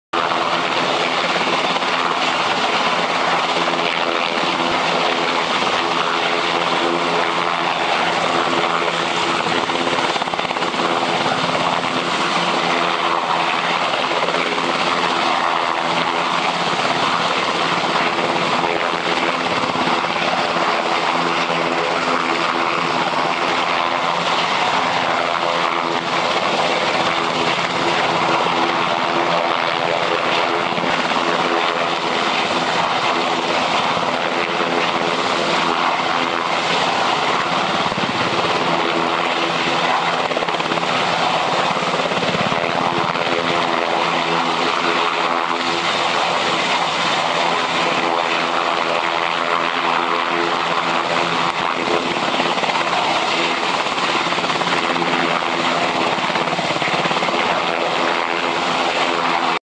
HM CoastGuard Helicopter Rescue Team Training
The S-92 is multi-purpose helicopter powered by twin GE CT7-8A turboshaft engines and has an aluminum airframe and some composite components. The four-bladed fully articulated composite main rotor blade is wider and has a longer radius than the Sikorsky S-70.